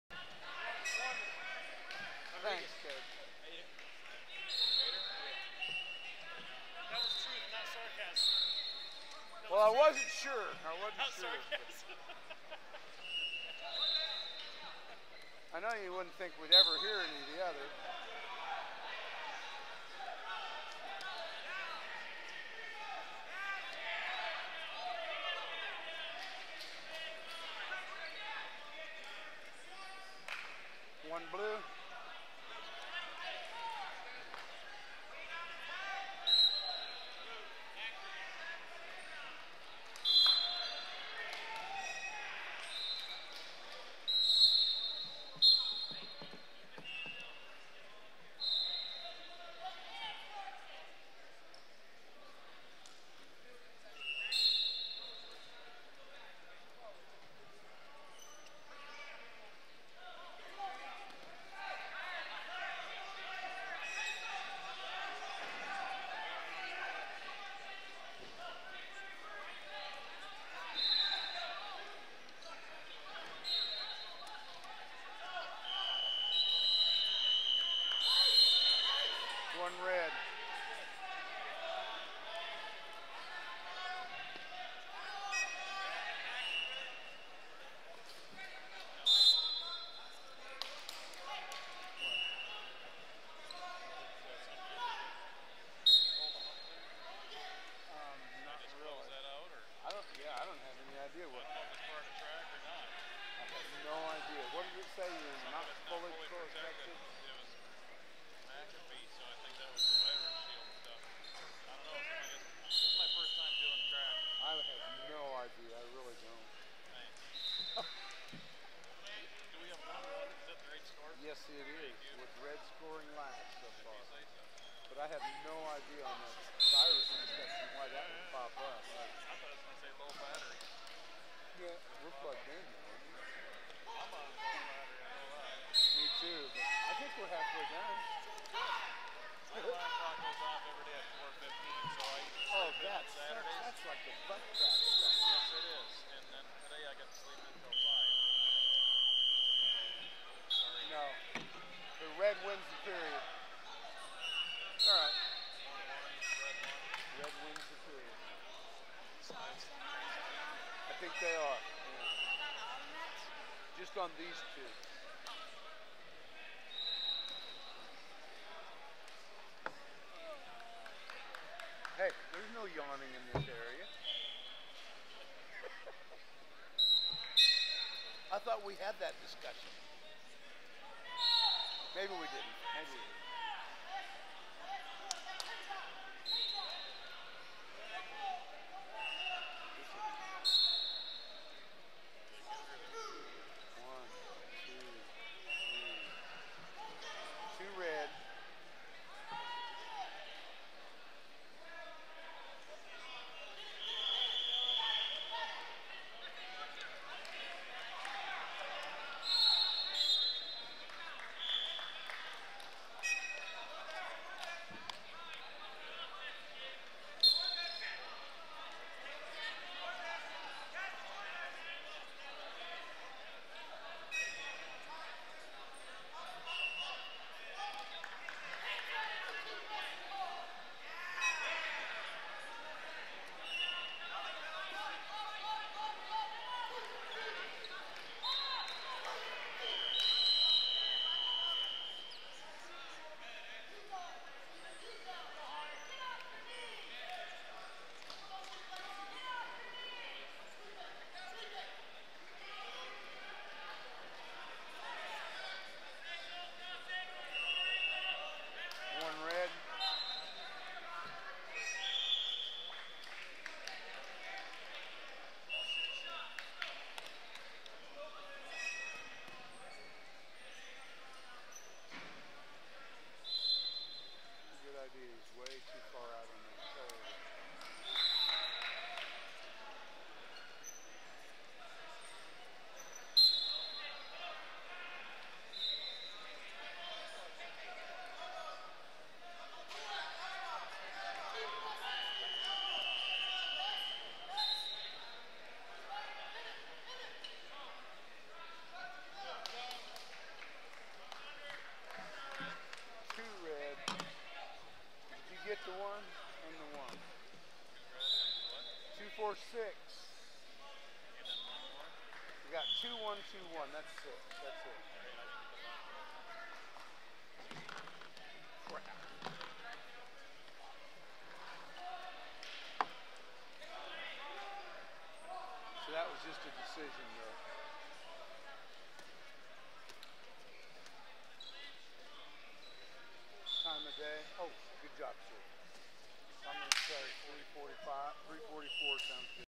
FILAJRFS: 63 KG / 138.75 lbs: Steven Rodrigues (Illini WC) vs. Taylor Moeder (HRWA)
Video By USA Wrestling. Check out the Semifinal action at the 2012 Junior Nationals - FILA - FS in the 63 KG / 138.75 lbs division with Steven Rodrigues of Illini WC vs. Taylor Moeder of HRWA. TheMat.TV